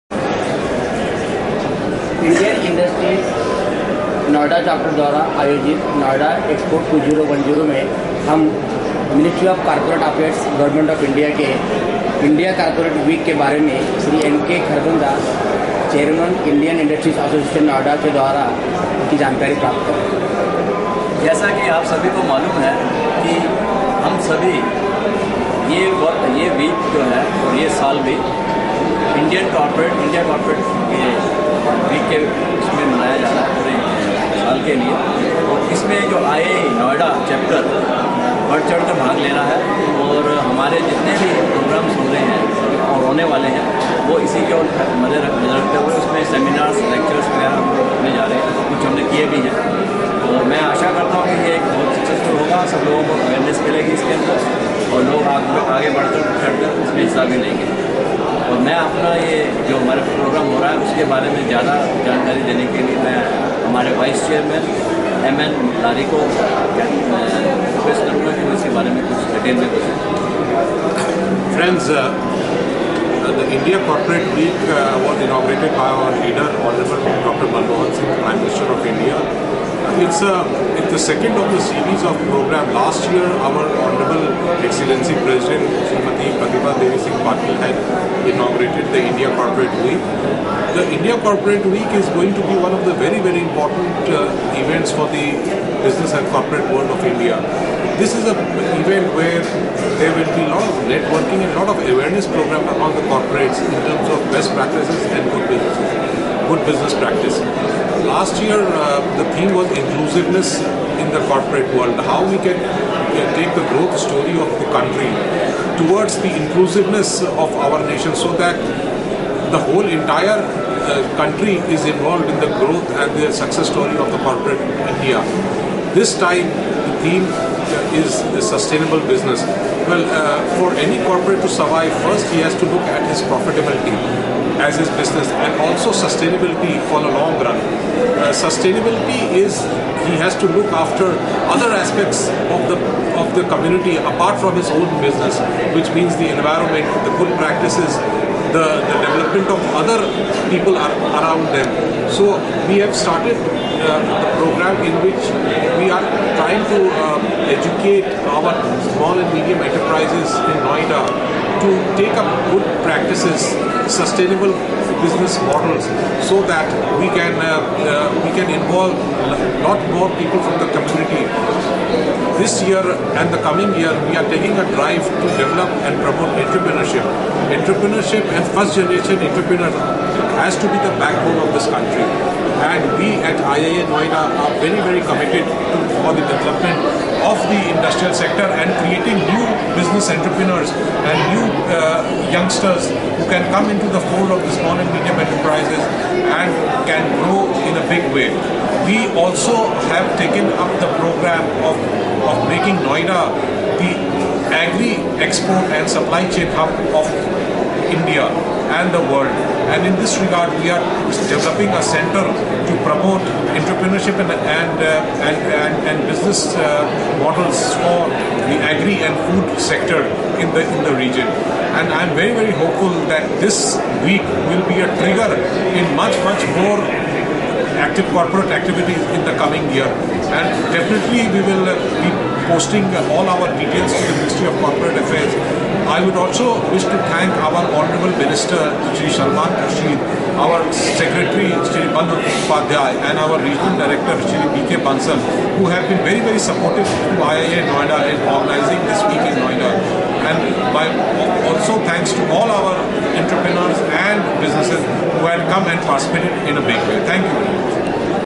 Ministry of Corporate Affairs Participates in IIA Noida EXPO 2010
Columbia Participates in IIA Noida Expo 2010 briefing by IIA Noida Chapter Chairman Shri N K Kharbanda and Shri M N Lari, Vice Chairman on 18/12/2010...